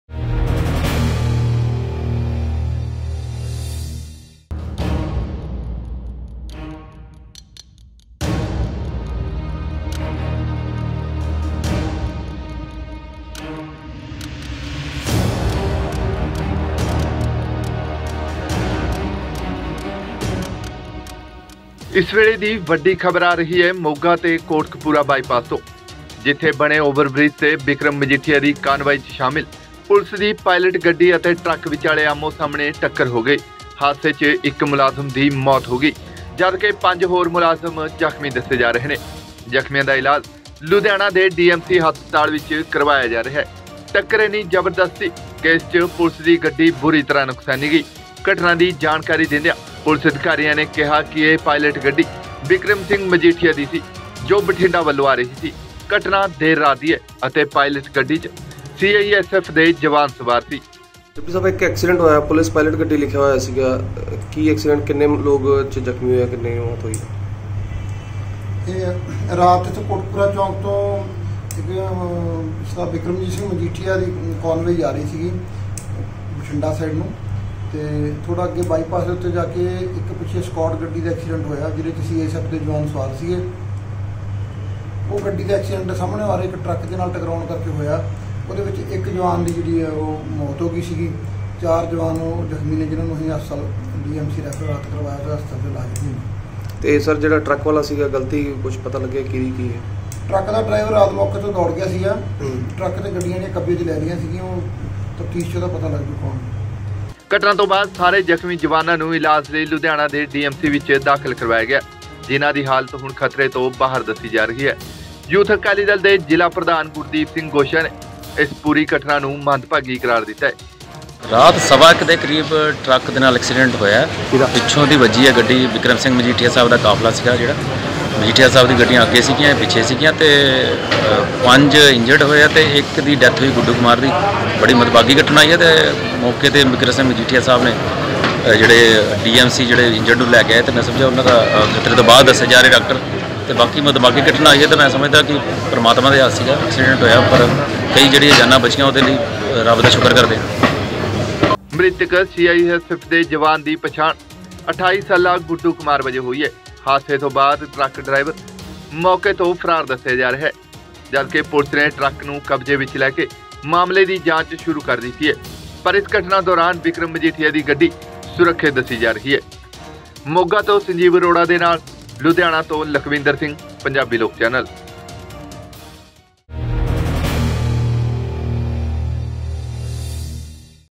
Big Breaking ਬਿਕਰਮ ਮਜੀਠੀਆ ਦੀ ਗੱਡੀ ਹਾਦਸੇ ਦਾ ਸ਼ਿਕਾਰ, ਮੌਤ
bikram majithiya ,accident ,